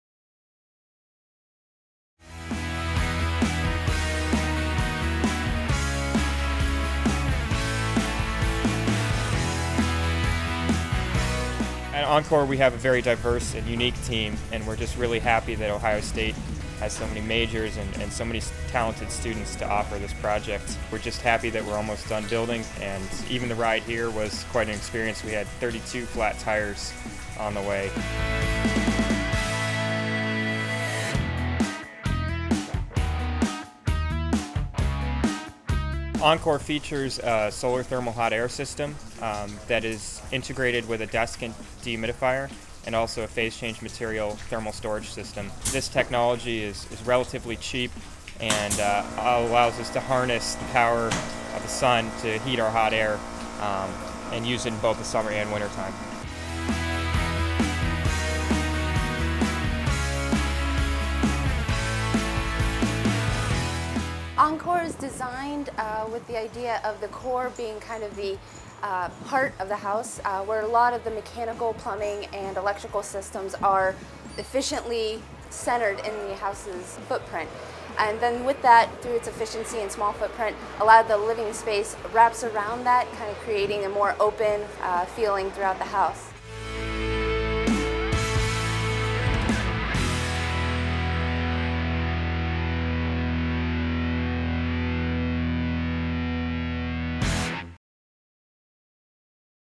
Ohio State Virtual Tour - Solar Decathlon 2011
Hear straight from the collegiate students from Ohio State about their design and strategy behind their entry in the U.S. Department of Energy Solar Decathlon 2011.

The U.S. Department of Energy Solar Decathlon challenges collegiate teams to design, build, and operate solar-powered houses that are cost-effective, energy-efficient, and attractive. The winner of the competition is the team that best blends affordability, consumer appeal, and design excellence with optimal energy production and maximum efficiency.